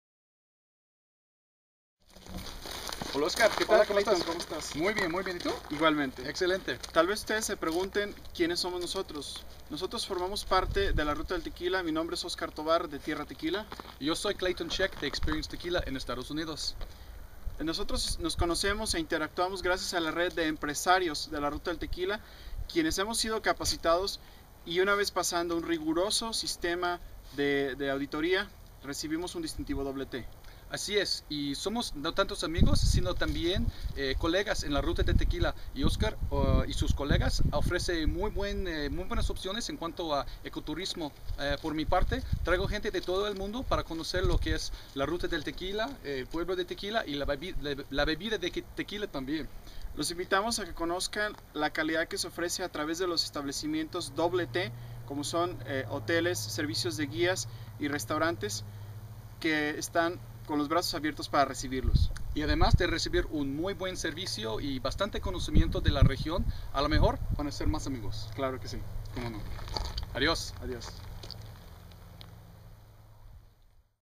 Dos empresarios nos hablan de La Ruta del Tequila
Con objeto de poner en valor la Ruta del Tequila como uno de los atractivos turísticos del país y planearla como un producto turístico de Jalisco con gran potencial, pedimos a dos empresarios (Oscar de Tierra Tequila & Clayton de Experience Tequila) explicarnos su interés por ofrecer servicios de calidad en esta Ruta.